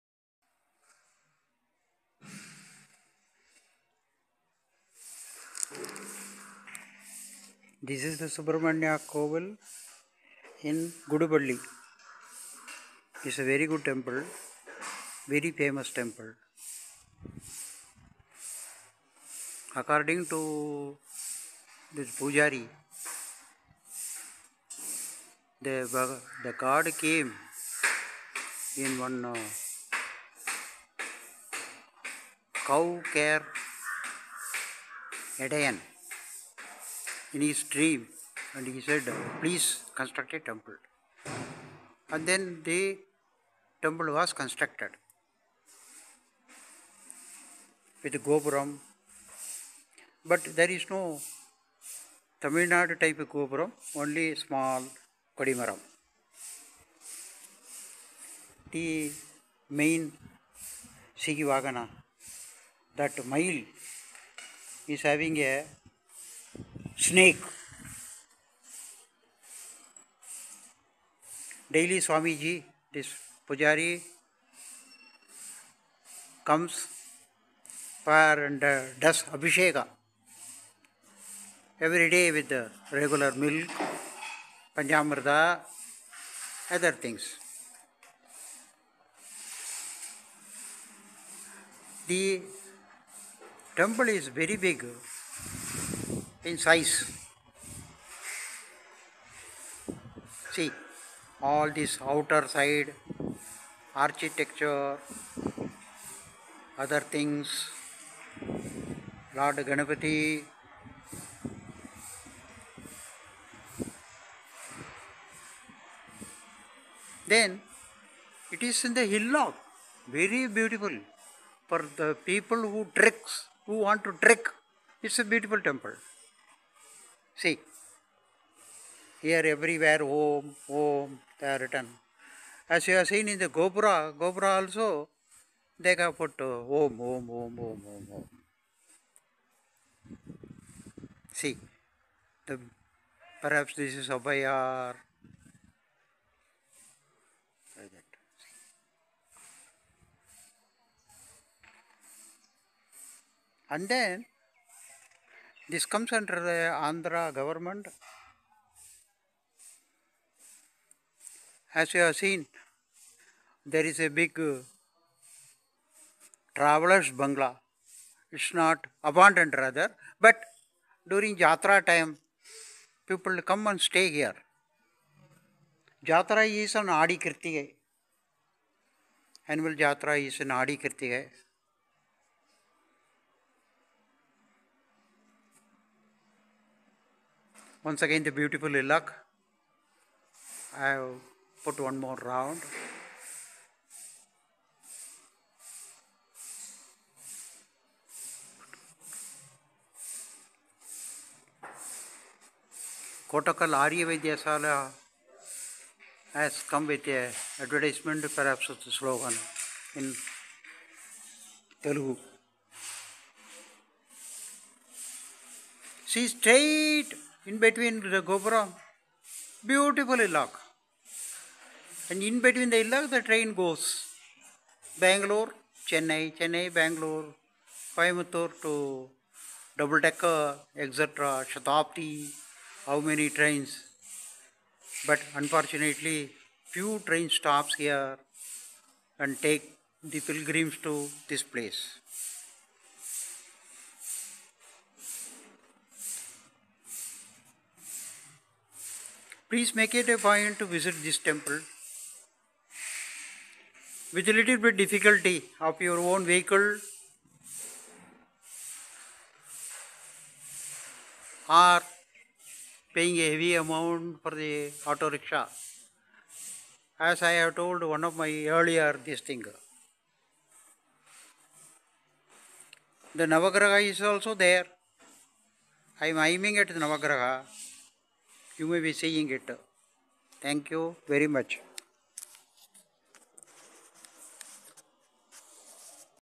5 April 2019 GUDUPALLE ( LONELY MURUAN)
Video from Meena Venkatesan AND EXCLUSIVELY FILMED BY BLOGGER NARAYANAN VENKATESAN IN LORD SUBRAMANIA SWAMY GUDY IN GUDUPALLE A,P ON 2/4 TUE. THE TEMPLE IS IN A HILLOCK IN A LONELY PLACE SURROUNDING PART OF YELAKHAI HILLS AND JUNGLE. VERY BEAUTIFUL CLEAN TEMPLE INDEED. GOT GANAPATHHY, NAVAGRAHA ETC. THERE ARE NO SIVA AND PARVATHY , PROVING A TAMIL VERB "KUNDRU THORUM ADOOM KUMAAARA" THIS SWAMY ALSO CAME IN A SHEPARD DREAM AND ADVISED HIM TO CONSRUCT A TEMPLE FOR HIM.
THE ARCHAKHA COME FROM GUDUPALLY VILLAGE INA CAR AND DO ABISHEK/ AARTI ETC.OPENS ATOUND 8  TO 8.30 AM REMAIN UPTO 1AM . DURING ADI KRITIGAI ANNUAL JATRA IS BEING HELD.
NO BUS TILL THE TEMPLE U HAVE TO DEPEND UPON PRIVATE AUTO AND IN MONDAYS SHARE AUTOS.
BUS SERVICE IS FROM KUPPAM TO GUDEHALLI ONE K.M FROM GUDEPALLY STATION
OM MURUGA
OM VADIVELA
OM VALLI MANALA